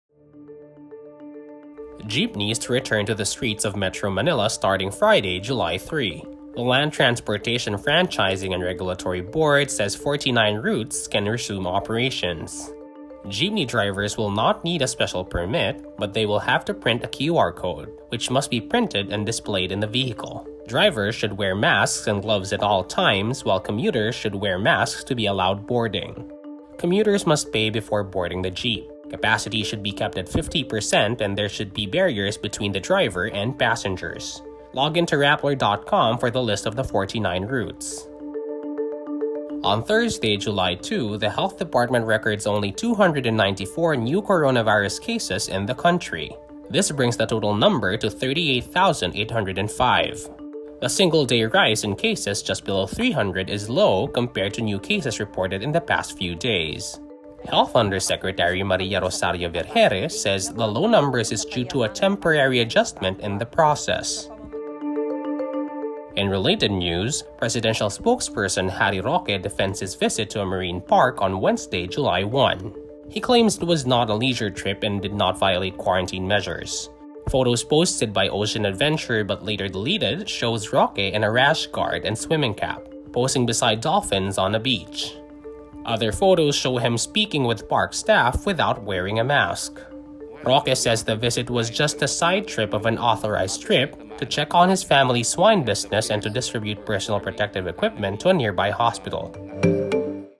LTFRB allows 49 routes for traditional jeepneys starting July 3
More videos on Rappler Jeepneys are to return to the streets of Metro Manila starting Friday, July 3. The Land Transportation Franchising and Regulatory Board says 49 routes can resume operations. Read